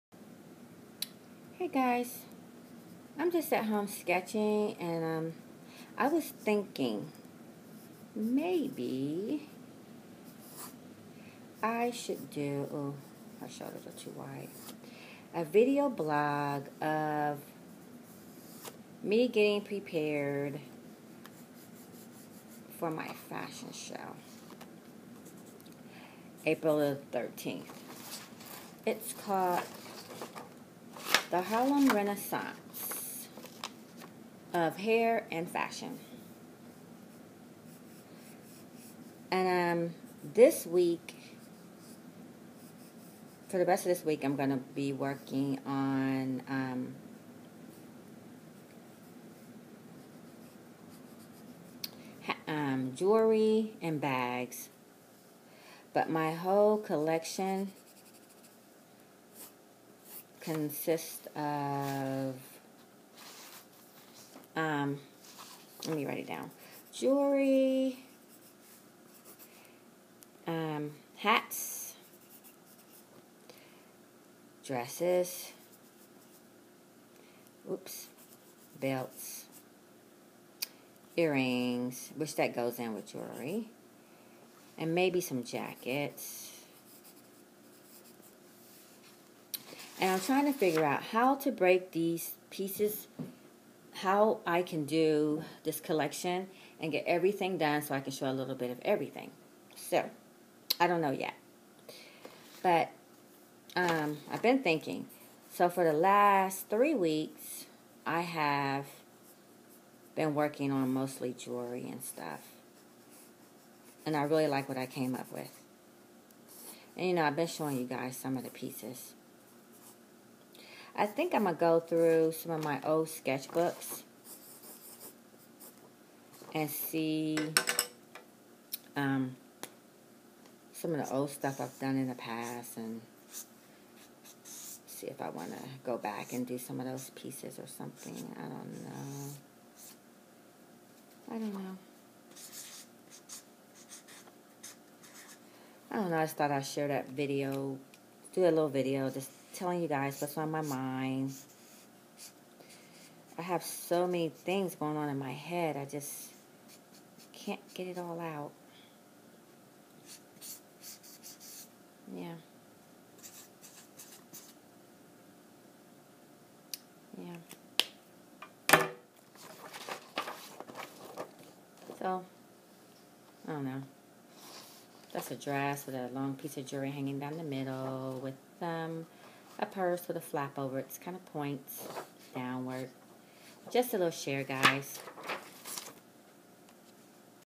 Video dairy 2/192013 part 1
A dairy of my ups and down getting ready for April 13 show